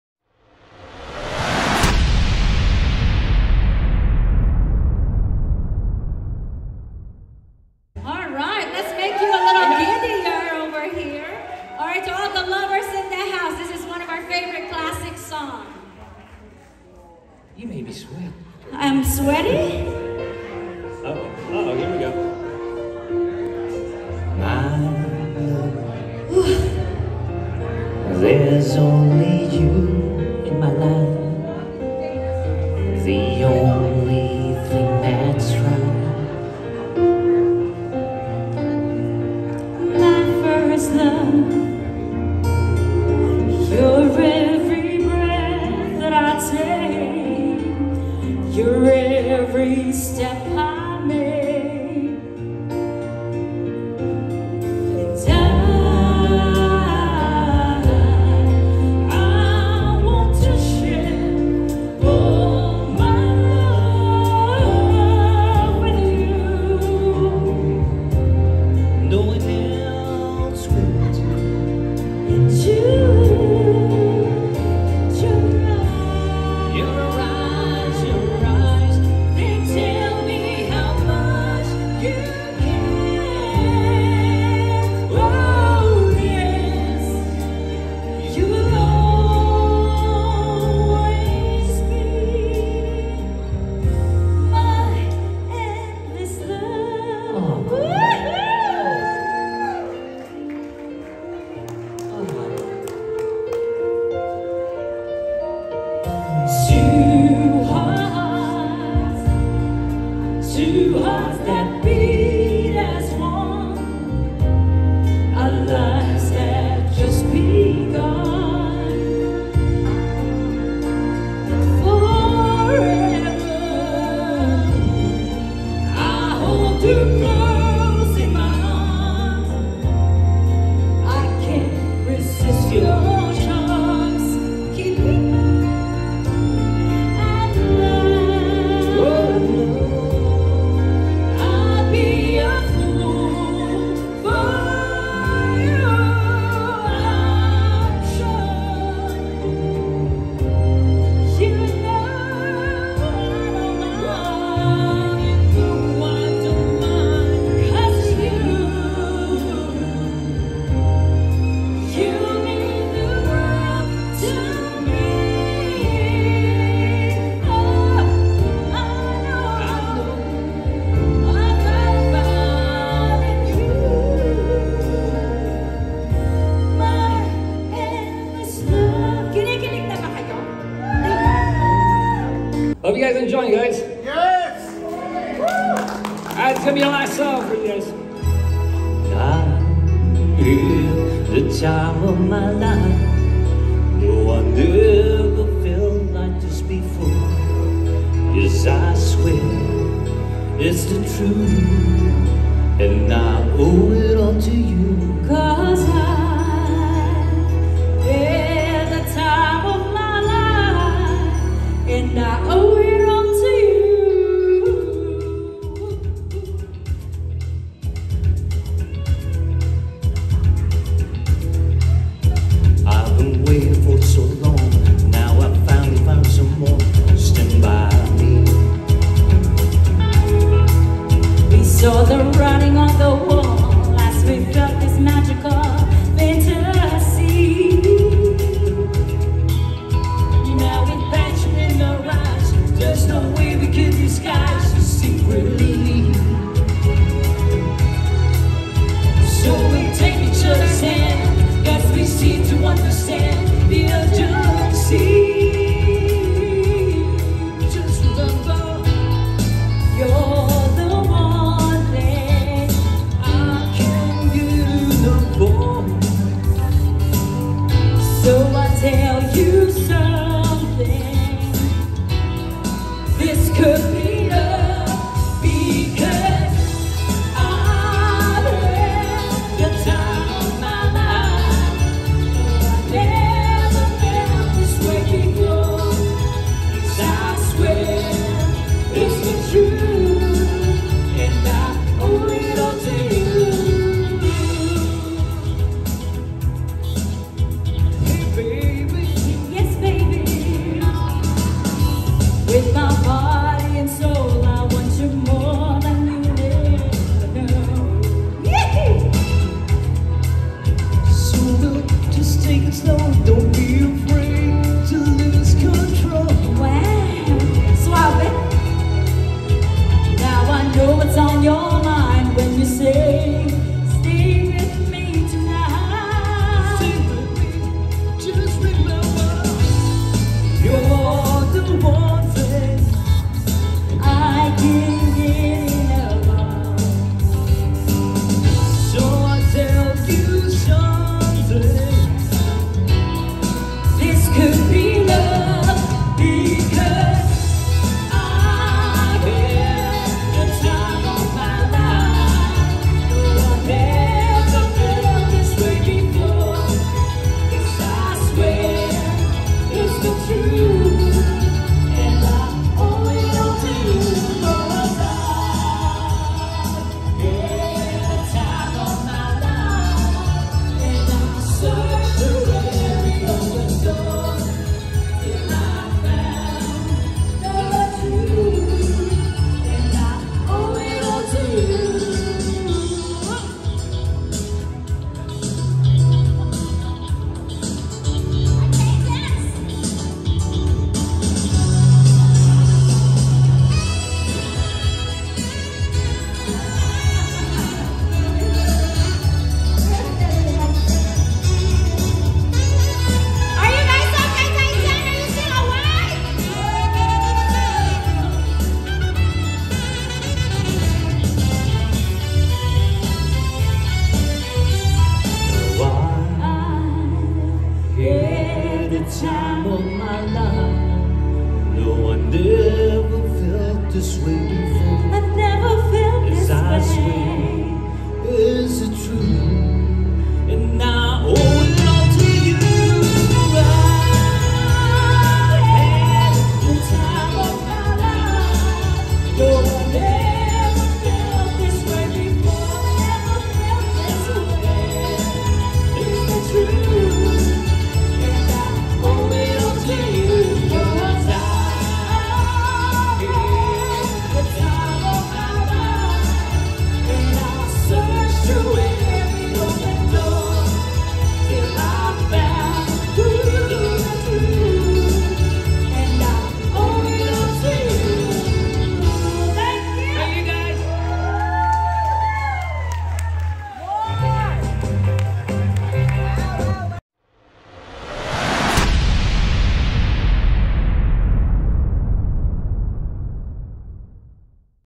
Opening for The King: Elvis Elvis Elvis Part 2 @elvisp85 Ryan Roth
Electric Night: Cats and Ants Ignite the Stage for Ryan Roth @elvisp85 , the Ultimate Elvis Impersonator, with the Comeback Special Band!